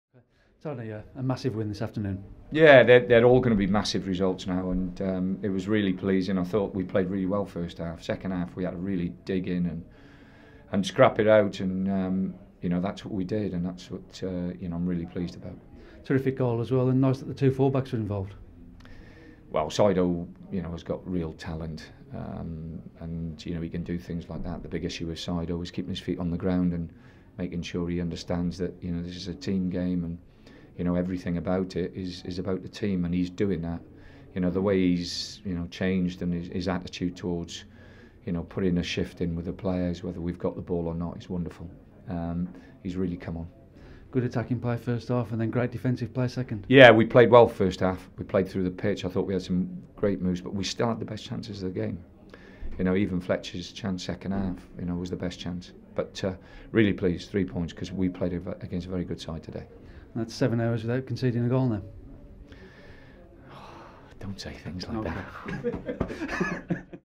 Tony Pulis is interviewed after Albion's 1-0 Premier League win over Southampton
West Bromwich Albion head coach Tony Pulis reacts to his side's 1-0 Barclays Premier League victory over Southampton at The Hawthorns.